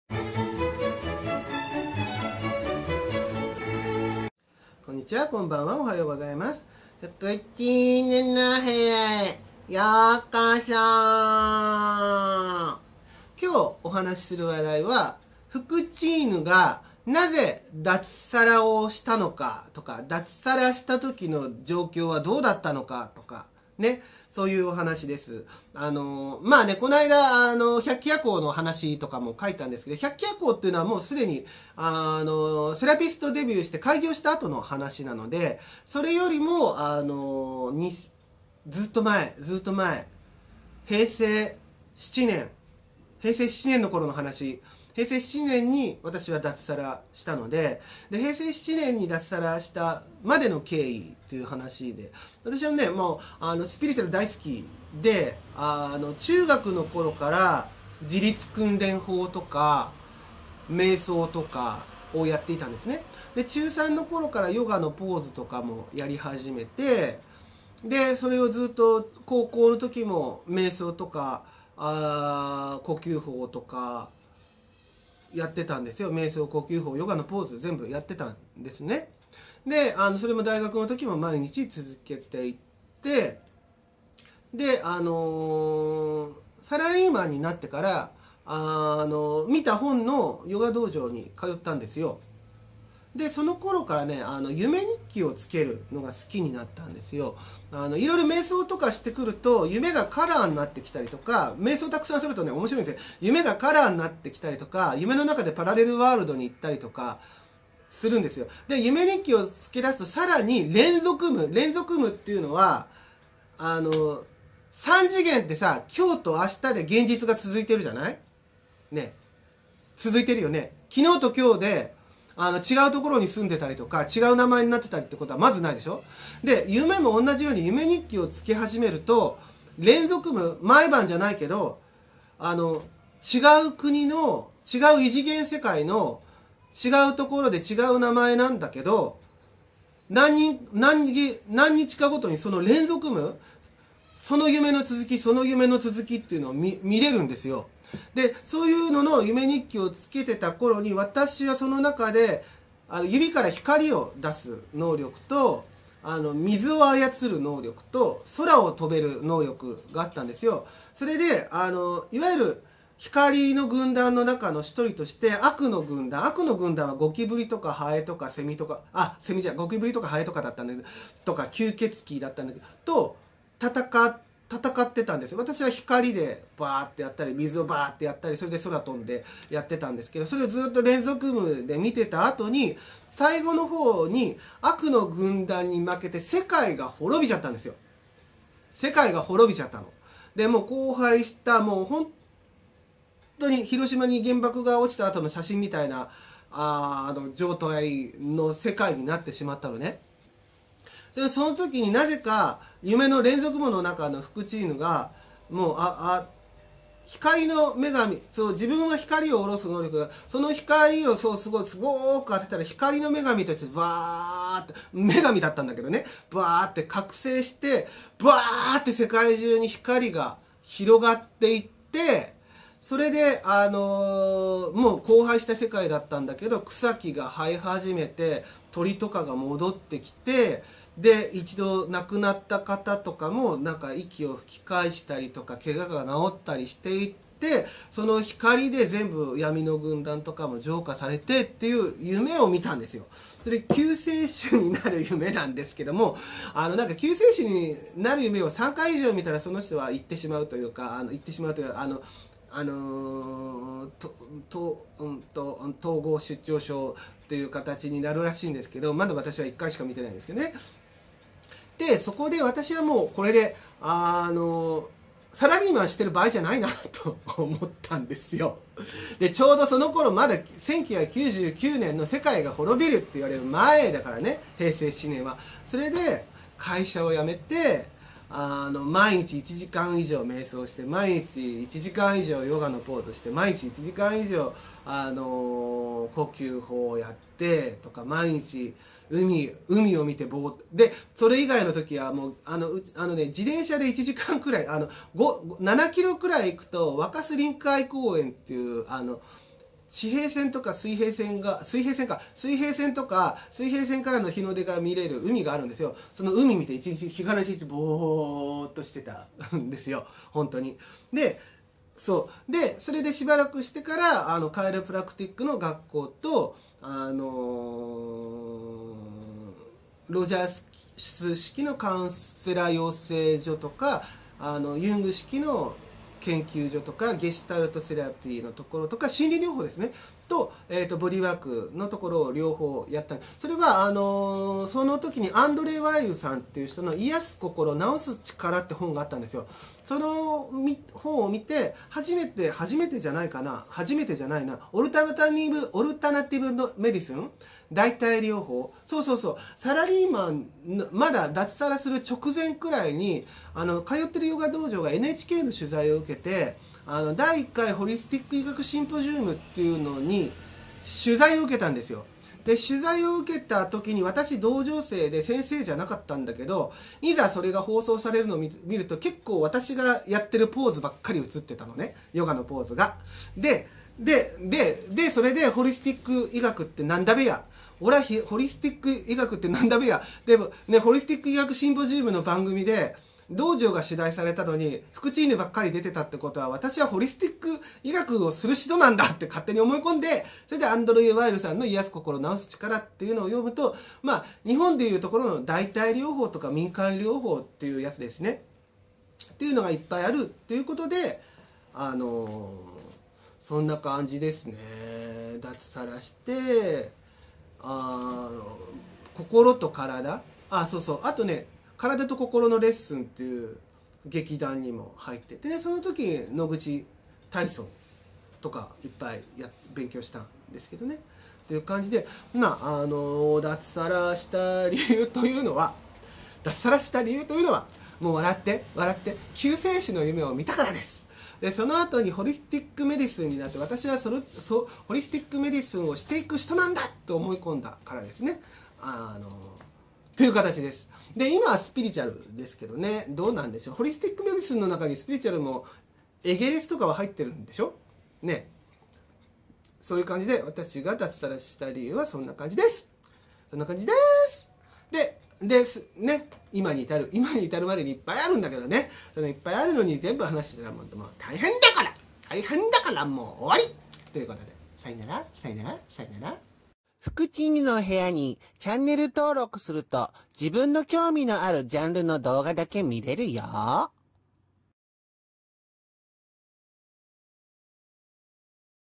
光と闇の戦いの連続夢で光の女神の救世主として覚醒して脱サラした話
２０代のサラリーマンの頃に夢日記をつけていて、その夢日記の中の連続夢(何か月にもわたって夢の世界でつづきが見れる)で光の勢力と闇の勢力の戦いがあって、闇に世界が滅ぼされた後に光の女神の救世主に覚醒して荒廃した世界が生成した夢をみたので脱サラした話

関連動画
自己紹介もかねて百鬼夜行を見た後に生死の境をさまよって、目覚めさせられ開花した話

関連ブログ
日本の神様タロット　ⅩⅩⅠ　Ｔｈｅ　Ｕｎｉｖｅｒｓｅ

４．７　日本再生へのフィールドワークと祈り

フクチ～ぬの自己紹介